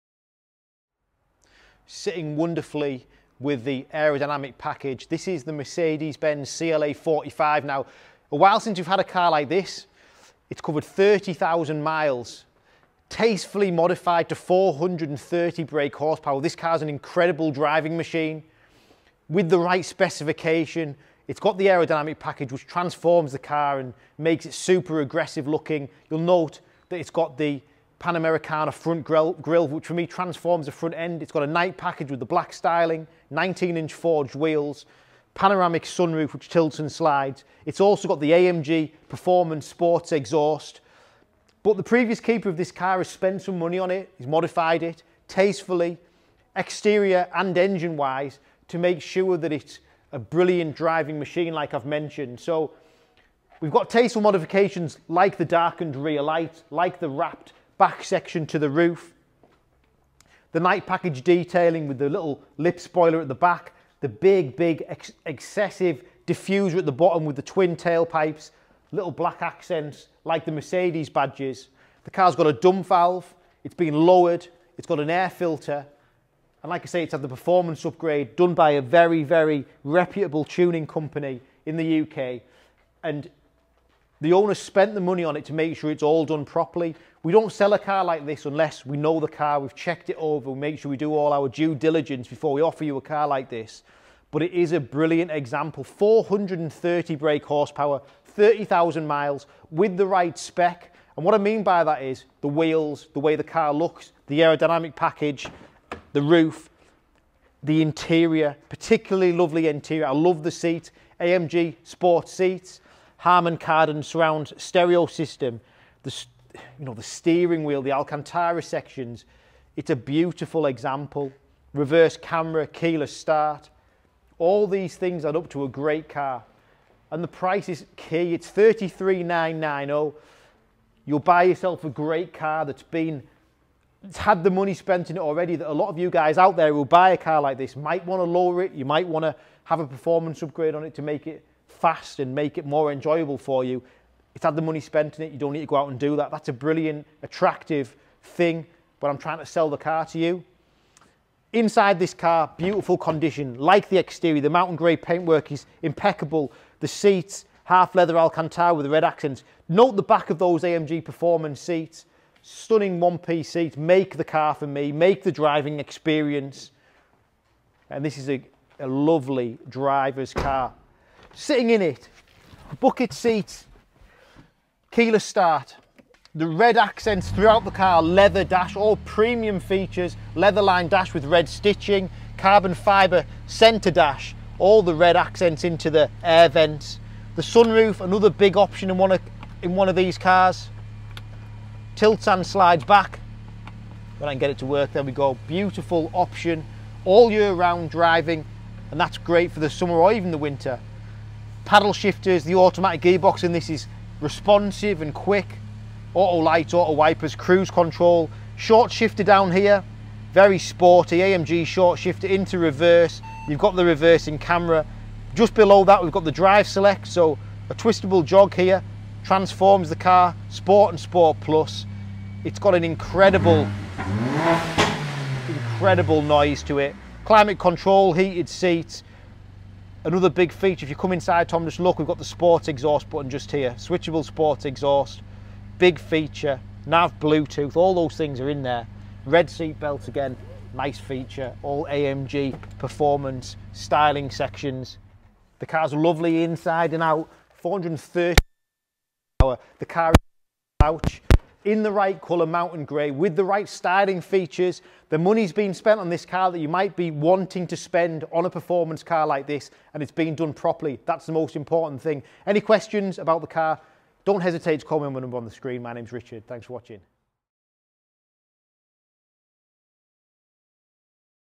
2017 Mercedes AMG CLA 45 4Matic
Already an impressive performance coupe, this special CLA 45 has been tastefully & expertly modified at the cost of the previous owner to enhance both power & style. Take a closer look 🔥

Only 30,290 miles with Full Service History. Live Priced at £33,990 - Buy & Reserve Online